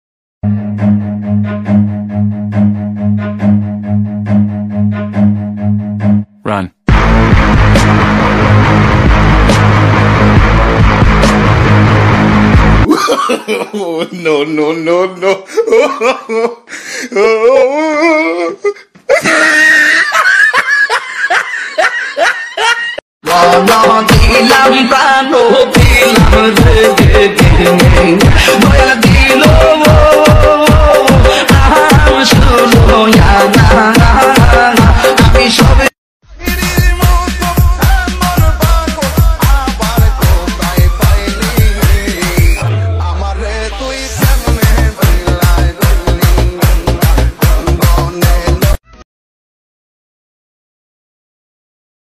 Drone funny video with a Farmer.
ভিডিও টা কেউ পারসোনাল ভাবে নিবেন না। এখানে কৃষক কে ছোট করা বা অপমান করা হয়নি। দয়া করে কেউ ভুল বুজবেন না। মজার জন্য ভিডিও করা